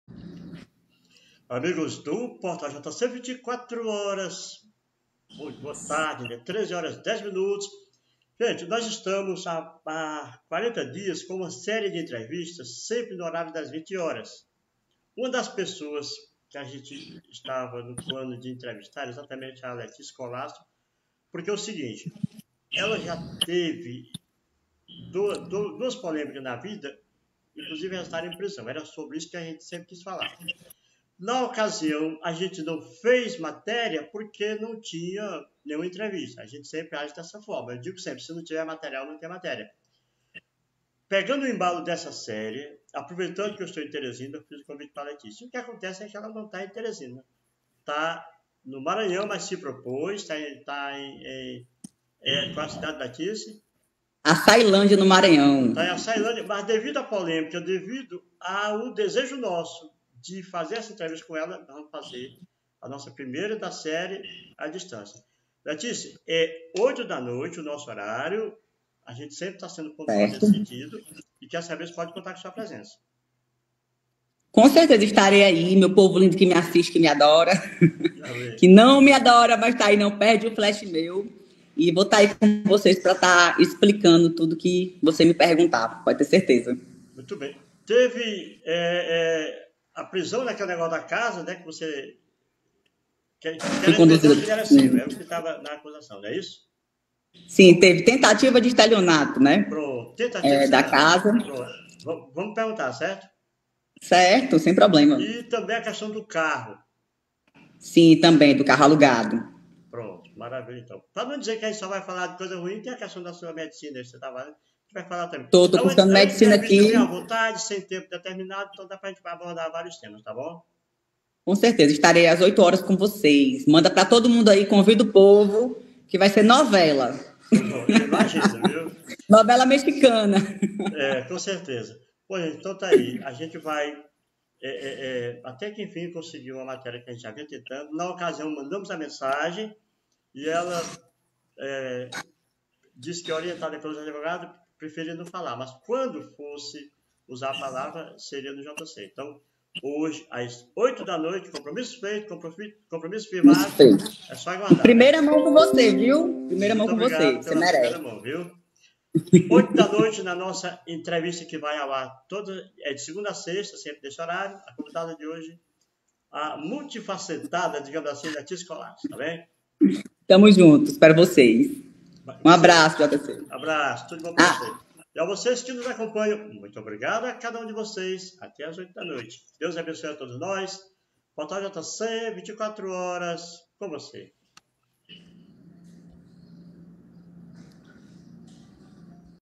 Imperdível! Letice Colasso é a entrevistada de hoje às 20h
Imperdível! Hoje, às 20h, não perca a entrevista exclusiva ao vivo com Letice Colasso no Facebook do JC24horas. Letice, renomada empresária do ramo de academias e estudante de medicina amplamente reconhecida em Floriano e região, será a entrevistada principal.
Nesta entrevista franca e reveladora, Letice se dispôs a esclarecer todos os detalhes e responder às perguntas dos internautas, oferecendo uma oportunidade única para entender os eventos recentes que a envolveram. Além disso ela vai falar sobre outros temas como atividade empresarial, saúde, entre outros que fazem parte do seu dia a dia. Não deixe de acompanhar esse momento importante e esclarecedor.